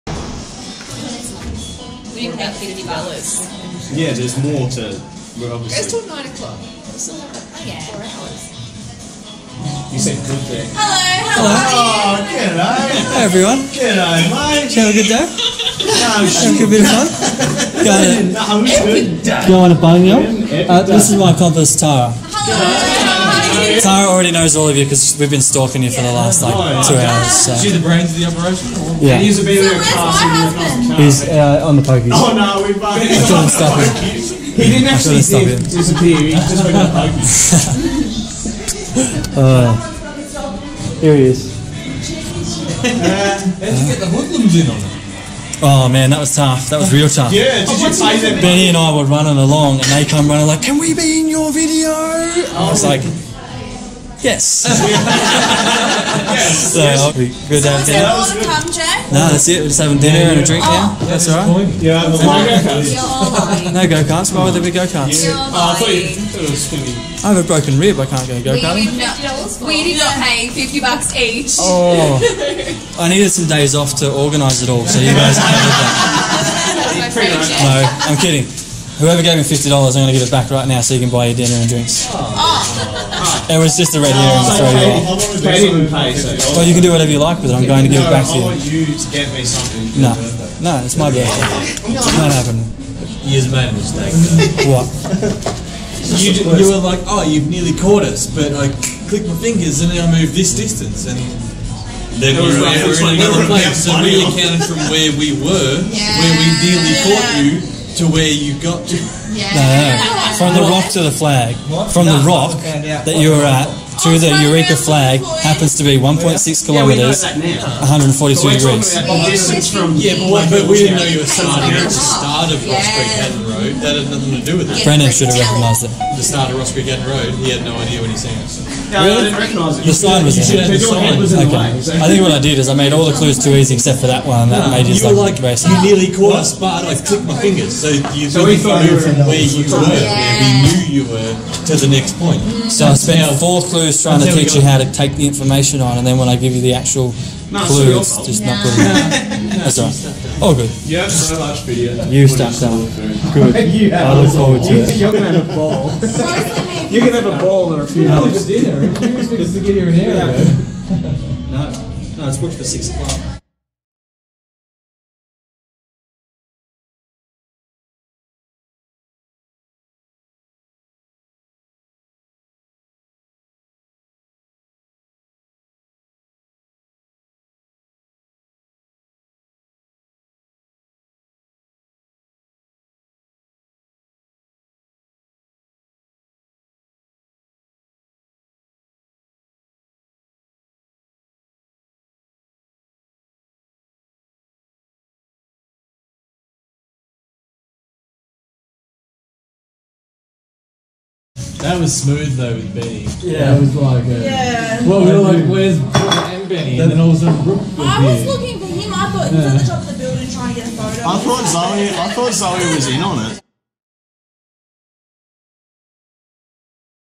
Amazing Chase Ballarat | Part 4
To celebrate my birthday I decided to set up a scavenger hunt around Ballarat.
Using some of Ballarat's beautiful monuments, and some creative video clues; I was able to surprise and challenge my friends.

Thanks to everyone for playing!